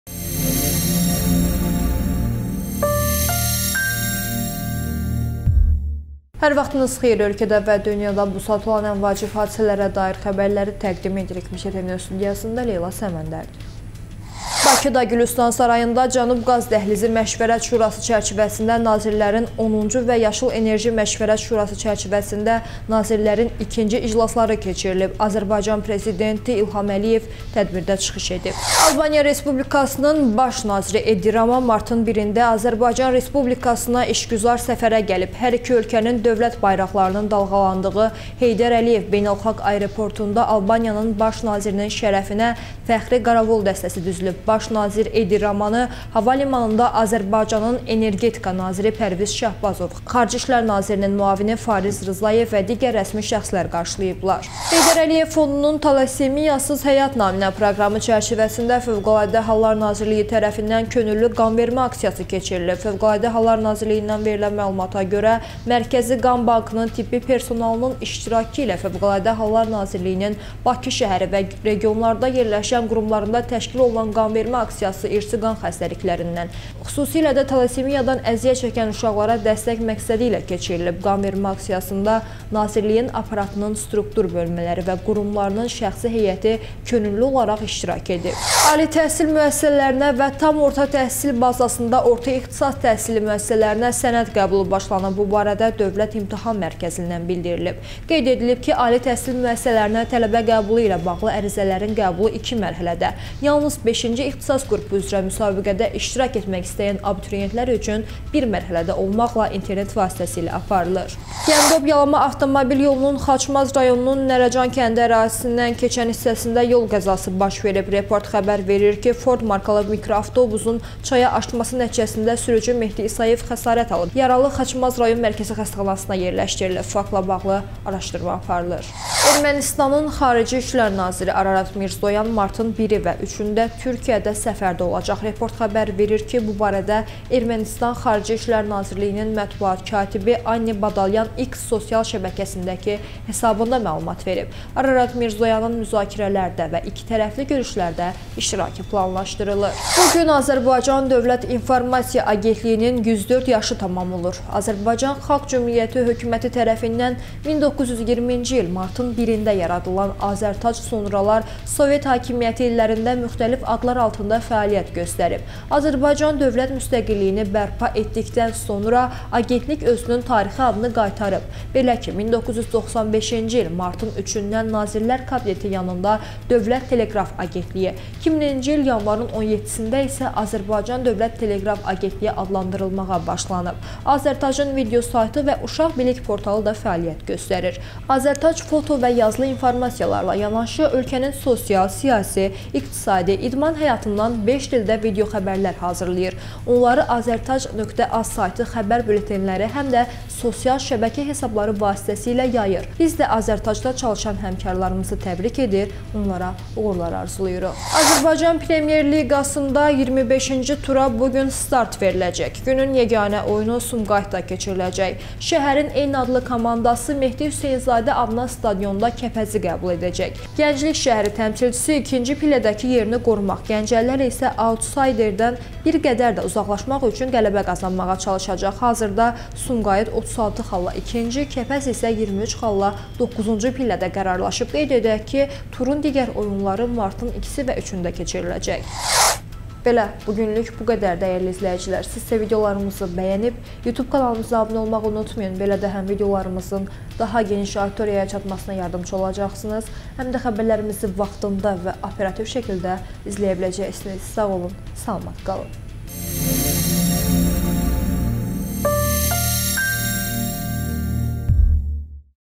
🔴 Ali və orta ixtisas təhsili müəssisələrinə sənəd qəbulu başlanıb
✅ Bakıda Cənub Qaz Dəhlizi və yaşıl enerji üzrə iclaslar keçirilir, İlham Əliyev tədbirdə çıxış edib
✅ Albaniyanın Baş naziri Edi Rama Azərbaycana işgüzar səfərə gəlib
✅ FHN-də qanvermə aksiyası keçirilib
✅ Xaçmazda mikroavtobus körpüdən aşıb, sürücü xəsarət alıb
✅ Ermənistan XİN: Mirzoyanın Türkiyədə ikitərəfli görüşlər keçirməsi planlaşdırılır
✅ Bu gün AZƏRTAC-ın 104 yaşı tamam olur
✅ Premyer Liqa: XXV tura bu gün Sumqayıtda start veriləcək